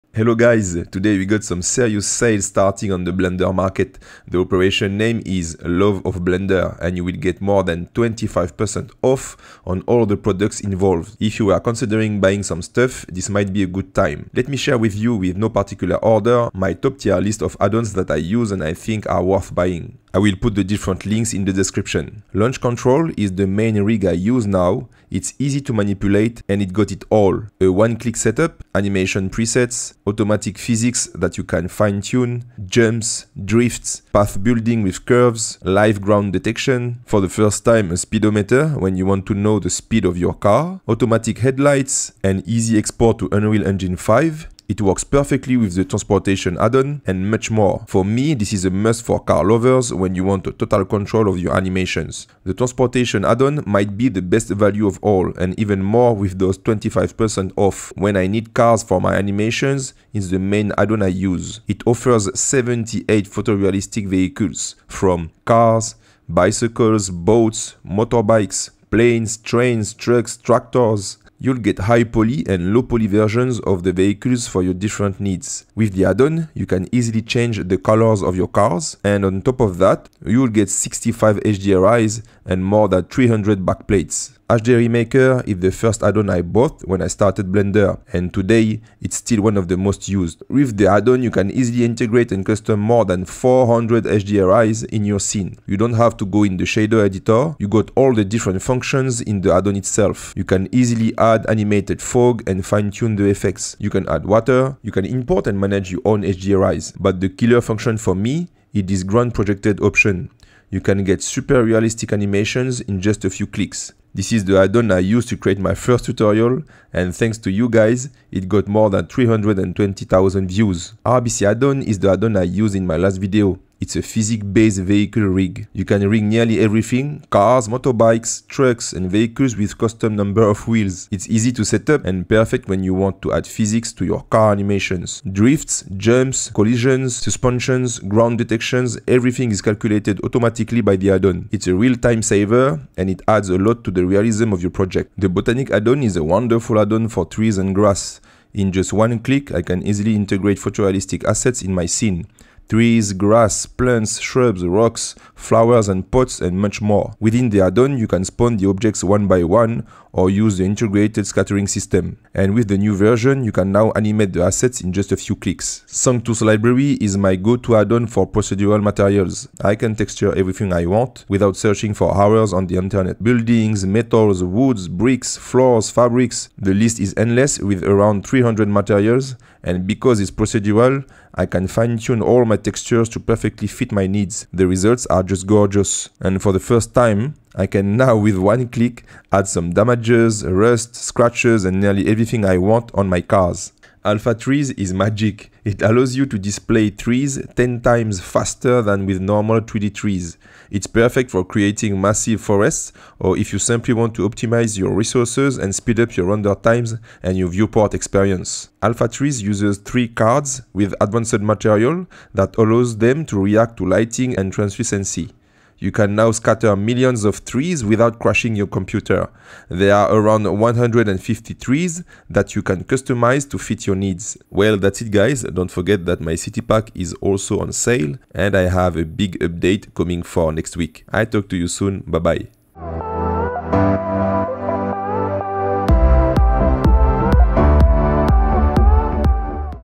Best Blender Add-ons on SALE | Love of Blender
Today, we got some serious sales starting on the Blender market. If you were considering buying some stuff, this might be a good time.

Timestamps
0:00 Intro
0:22 Launch control
0:52 Transportation add-on
1:24 Hdri Maker
2:00 RBC
2:24 Botaniq
2:45 Sanctus Library
3:13 Alpha trees
3:44 Love and more